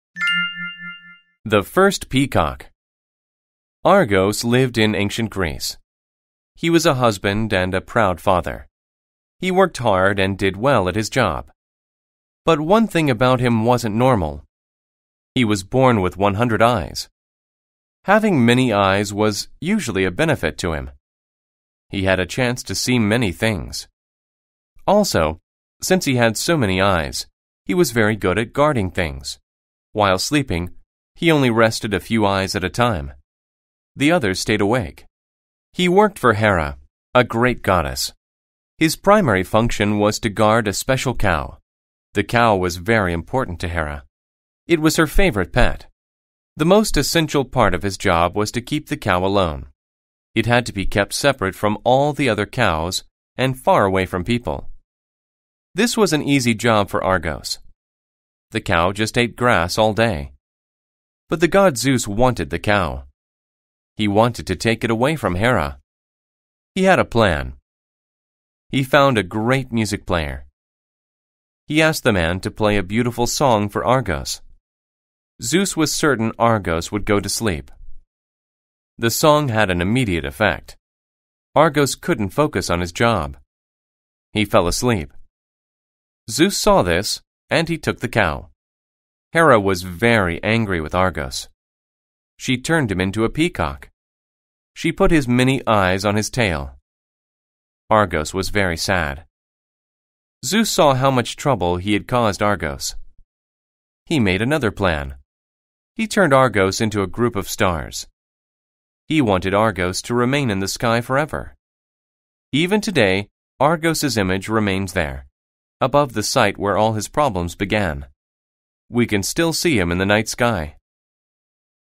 الطاووس الأول | قصة إنجليزية قصيرة جدا مترجمة للعربية
معنا في هذا الدرس قصة قصيرة جديدة مترجمة للعربية تحتوي على الكثير من الكلمات المهمة للمبتدئين، هذه القصة مترجمة من سلسلة كتب أهم 4000 كلمة في اللغة الإنجليزية.
أكون سعيدا عند اشتراكك في قناتي
وكذلك زيارة موقعي على الانترنت